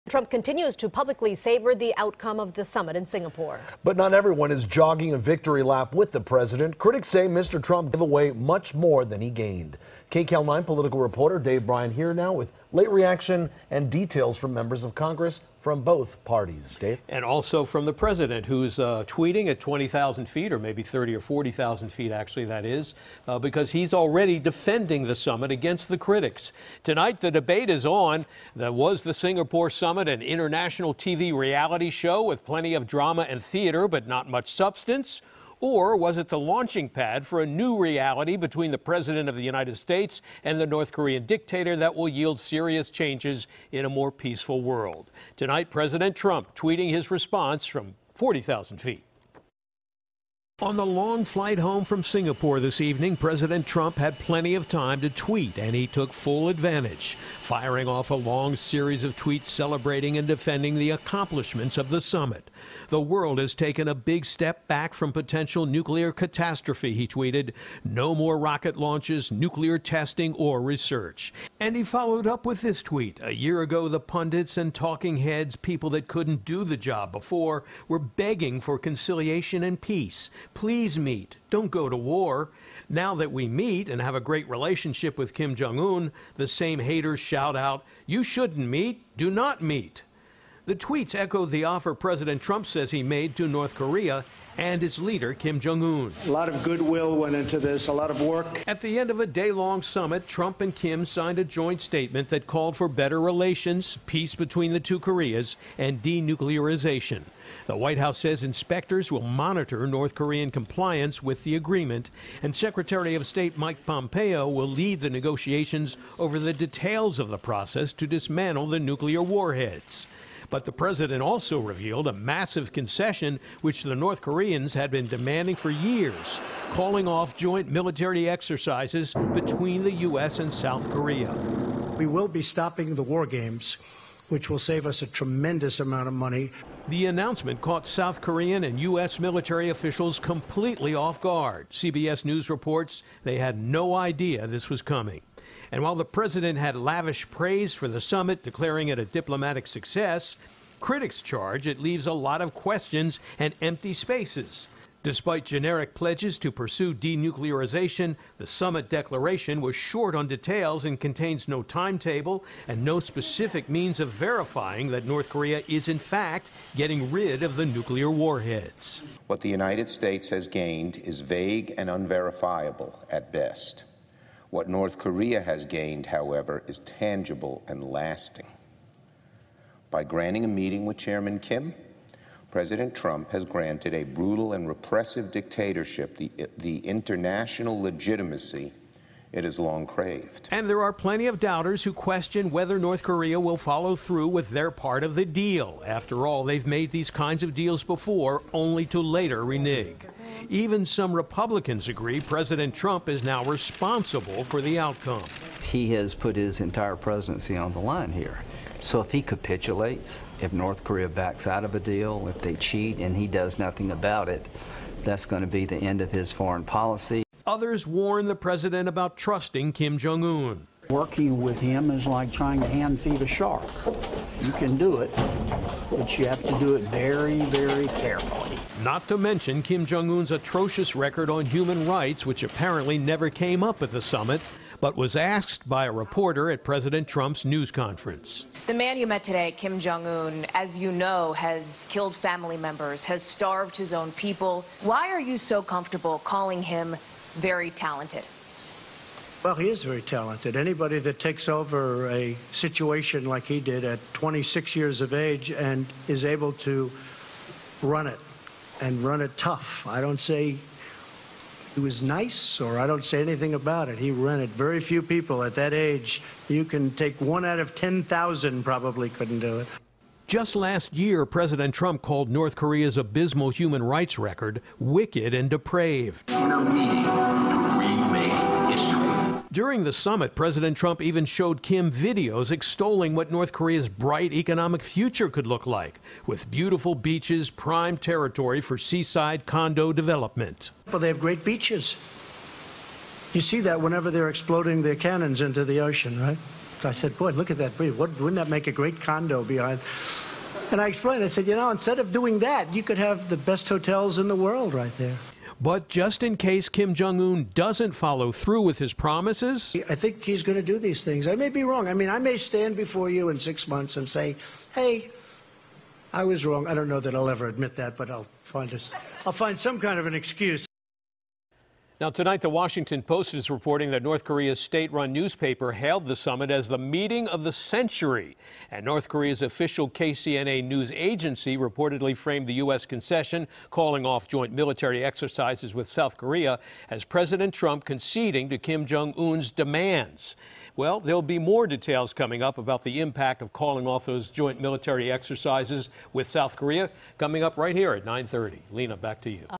President Takes Victory Lap After Meeting With North Korean Leader
But not everyone, including members of his own party, are so sure about making an agreement with a brutal dictator. Dave Bryan reports.